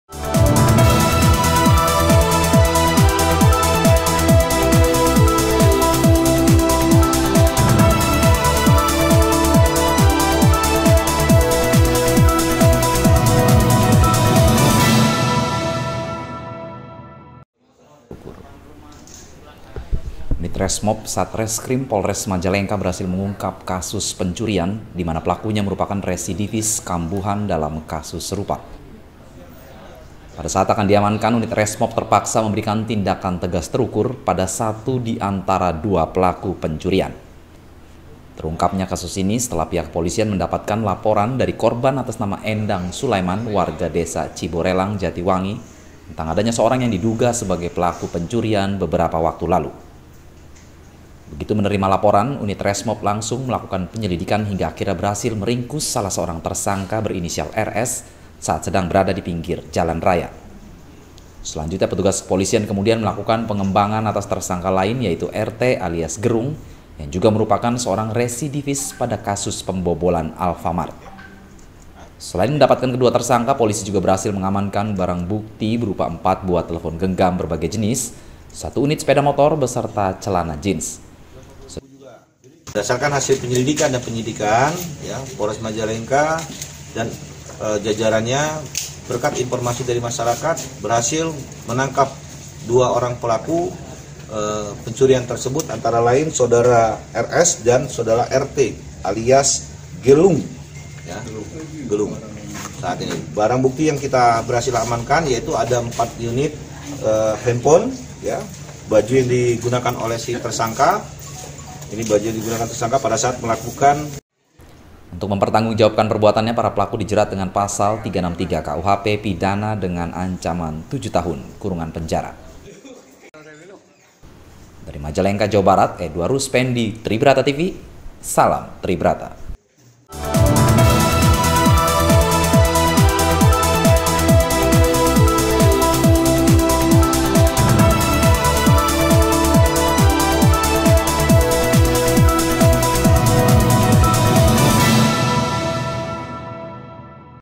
POLRES MAJALENGKA BERIKAN TINDAKAN TERUKUR KEPADA PELAKU PENCURIAN
UNIT RESMOB SATRESKRIM POLRES MAJALENGKA BERHASIL UNGKAP KASUS PENCURIAN DIMANA PELAKUNYA MERUPAKAN RESIDIVIS KAMBUHAN DALAM KASUS SERUPA. PADA SAAT AKAN DIAMANKAN  UNIT RESMOB TERPAKSA MEMBERIKAN TINDAKAN TEGAS TERUKUR  PADA SATU DIANTARA DUA PELAKU PENCURIAN TERSEBUT